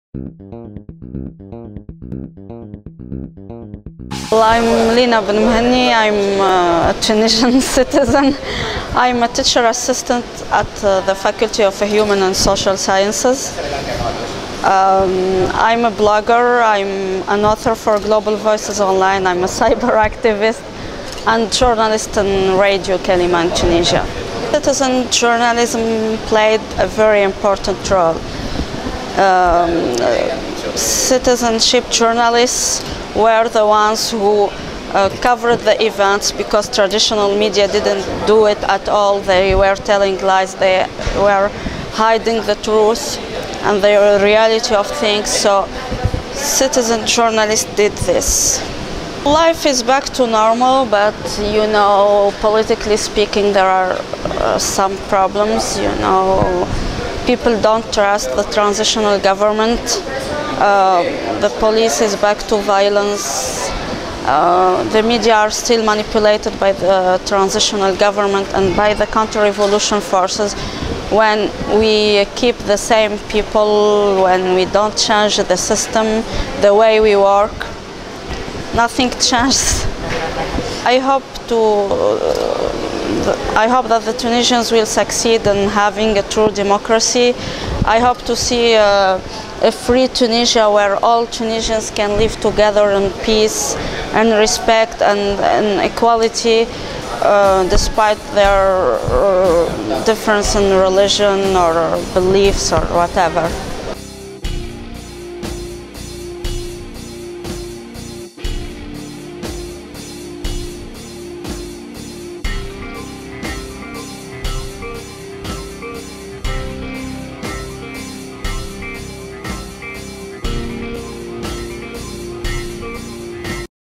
Lina Ben Mehnni y el importante papel de los periodistas ciudadanos en las revueltas de Túnez
Los perioidstas ciudadanos han jugado un importante papel en la Revolución de Túnez, fueron los primeros que cubrieron los acontecimientos porque los medios tradicionales no lo estaban haciendo. Además los medios tradionales mentían y escondian la realidad que estaba sucediendo.

Nos cuenta como todo está volviendo a la normalidad en Túnez pero lo que cuentan los politicos la gente no lo cree la policía vuelve a ser violenta y los medios de comunicación tradicionales vuelven a estar manipulados, en esta ocasión por el gobierno de transición...

Lina espera que con esta revolución se consiga un Túnez libre en el que todos los tunecinos y tunecinas puedan vivir en paz y respeto sin importar religiones, creencias ...